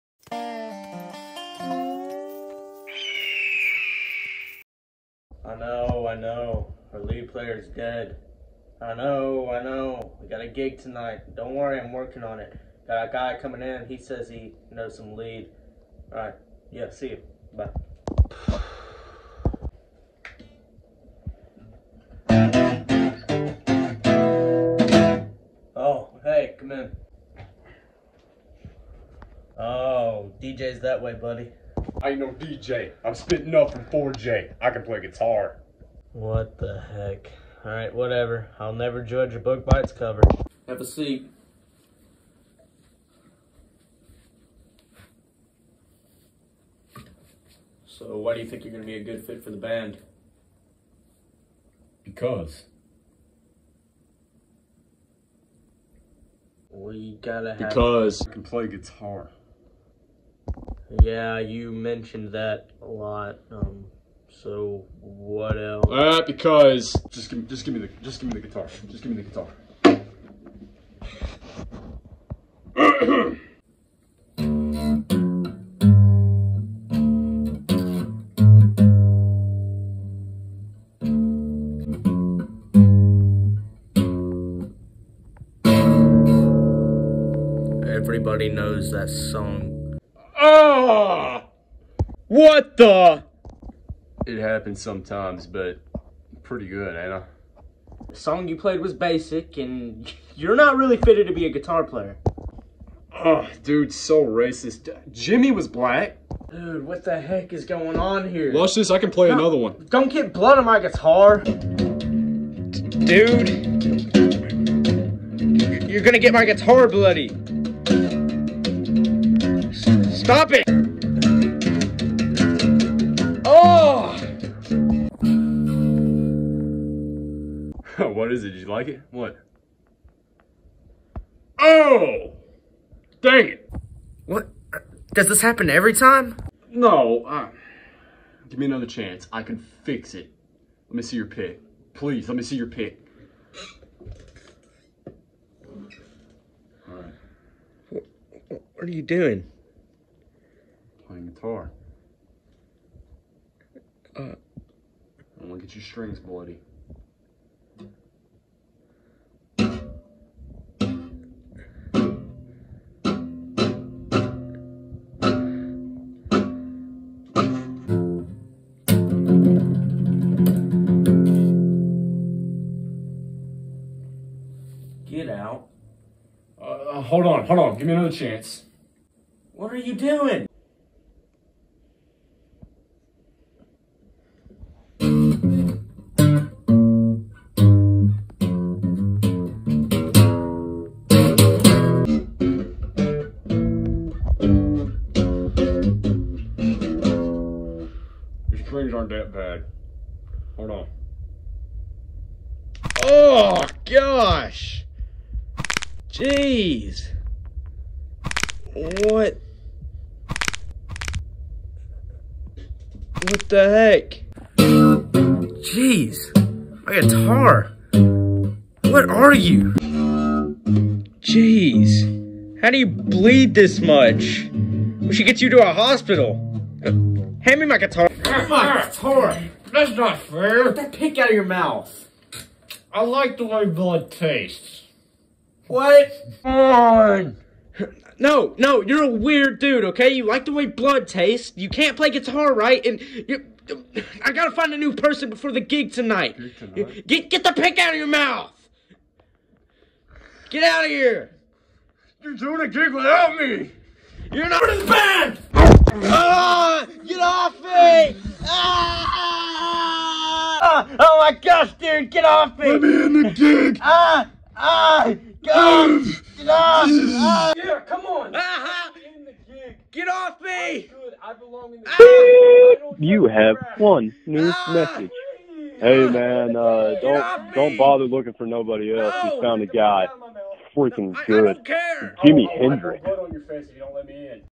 Pick in your mouth
A man needs to find someone before the gig tonight